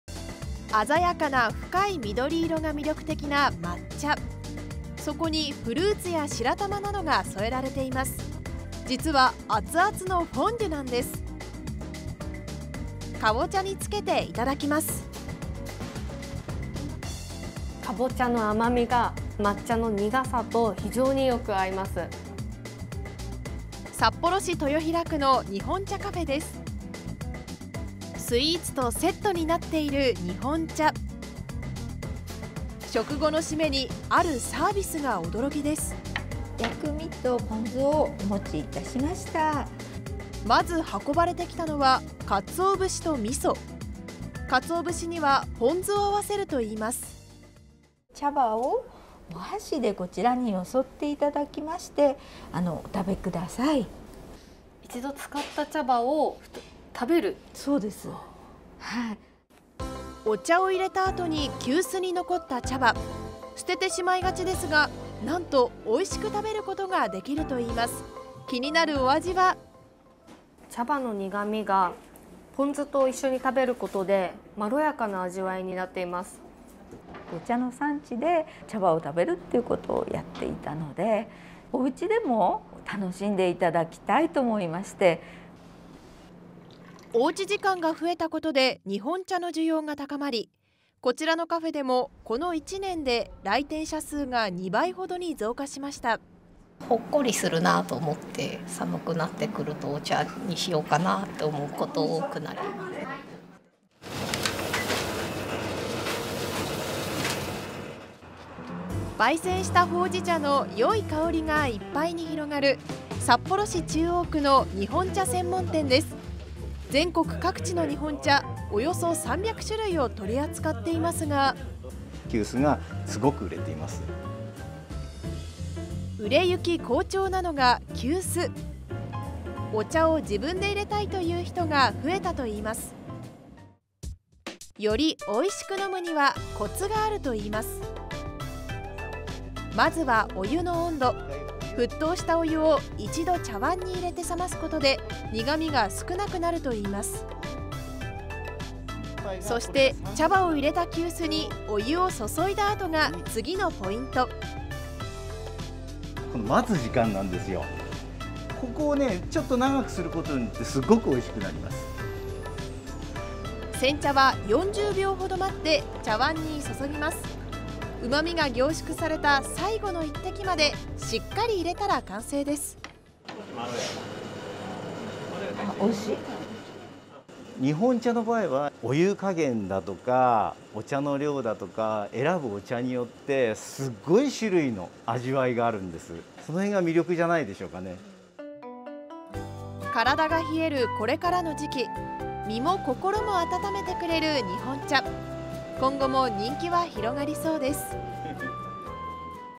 飲むだけ？いやいや、その後が美味 薬味とポン酢でいただく日本茶
スイーツとセットになっている日本茶。食後のしめの、あるサービスが驚きです。（2021年11月18日放送）